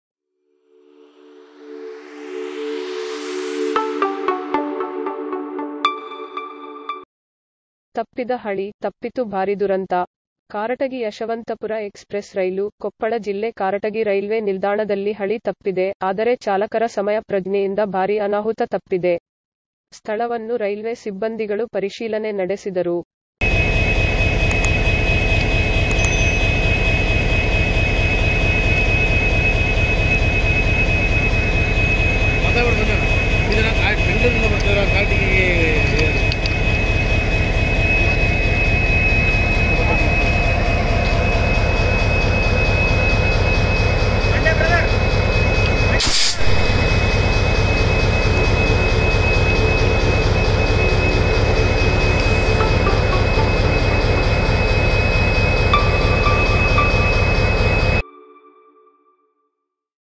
ತಪ್ಪಿದ ಹಳಿ; ತಪ್ಪಿತು ಭಾರಿದುರಂತ:
ಕಾರಟಗಿ  ಯಶವಂತಪುರ  ಎಕ್ಸ್ ಪ್ರೆಸ್ ರೈಲು, ಕೊಪ್ಪಳ ಜಿಲ್ಲೆ ಕಾರಟಗಿ ರೈಲ್ವೆ ನಿಲ್ದಾಣದಲ್ಲಿ ಹಳಿ ತಪ್ಪಿದೆ.ಆದರೆ ಚಾಲಕರ ಸಮಯಪ್ರಜ್ಞೆಯಿಂದ   ಭಾರಿ ಅನಾಹುತ  ತಪ್ಪಿದೆ.   ಸ್ಥಳವನ್ನು ರೈಲ್ವೆ ಸಿಬ್ಬಂದಿಗಳು ಪರಿಶೀಲನೆ ನಡೆಸಿದರು.